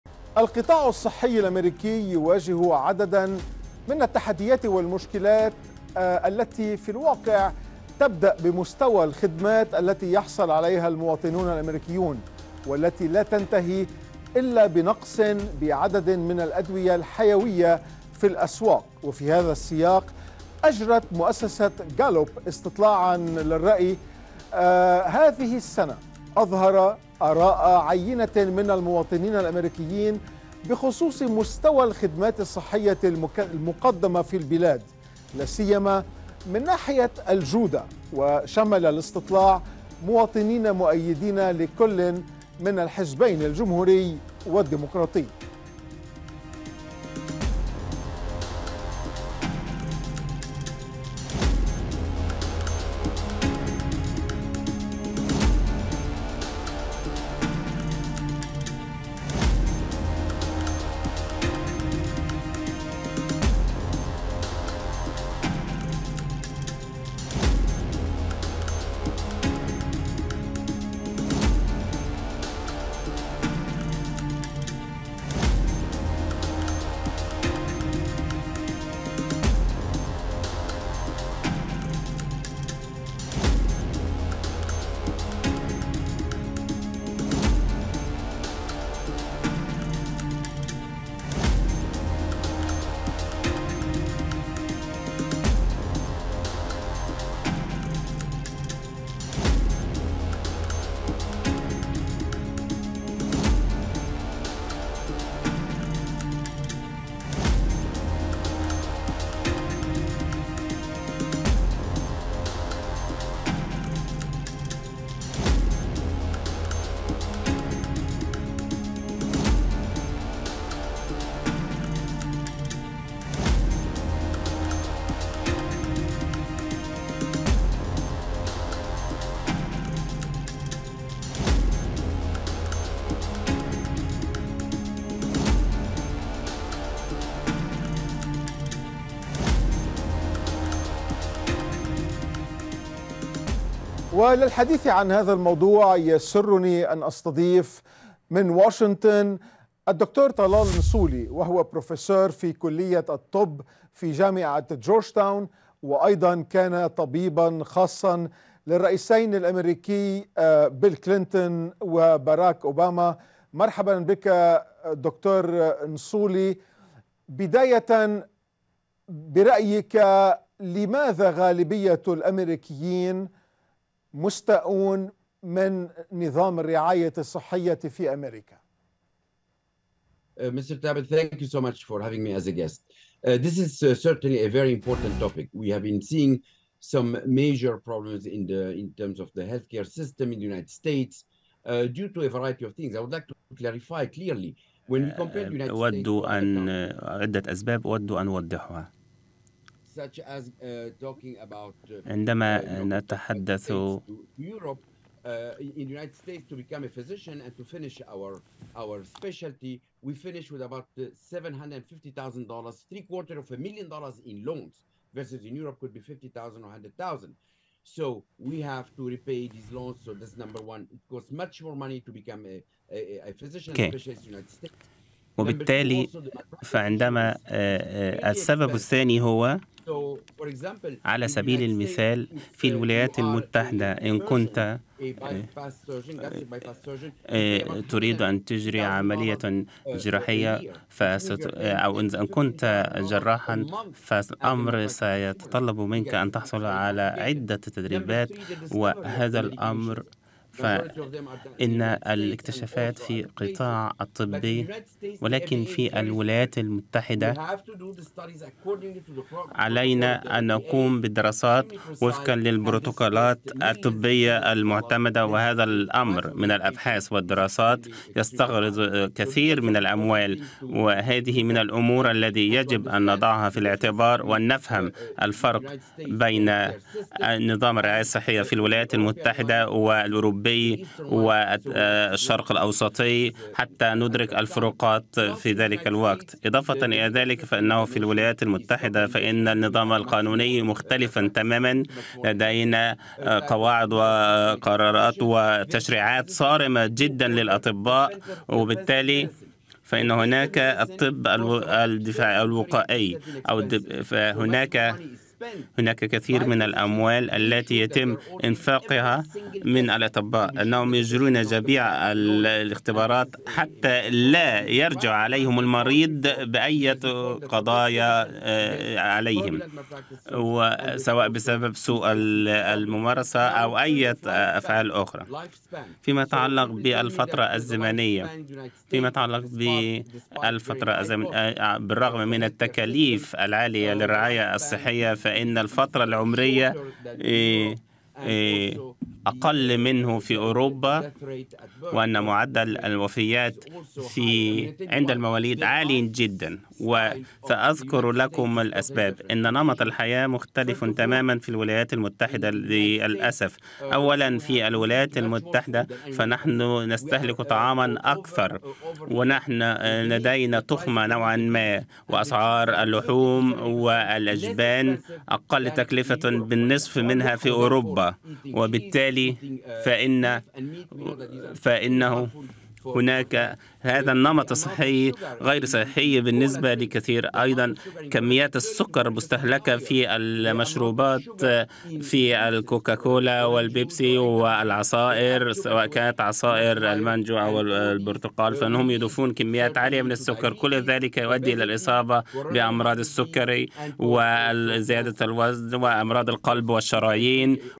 القطاع الصحي الأميركي ...خطوات حتى لا يدخلَ في غرفة الإنعاش
يواجه القطاع الصحي الأميركي عددا من التحديات والمشاكل لا تبدأ بمستوى الخدمات التي يحصل عليها الأميركيون ولا تنتهي بنقص عدد من الأدوية الحيوية في الأسواق.
وفي هذا السياق أجرت مؤسسة "غالوب" استطلاعًا للرأي هذه السنة أظهر آراء عينة من المواطنين الأميركييين بخصوص مستوى الخدمات الصحية المقدمة في البلاد، لاسيما من ناحية الجودة. وشمل الاستطلاع مواطنين مؤيدين لكل من الحزبين الجمهوري والديموقراطي.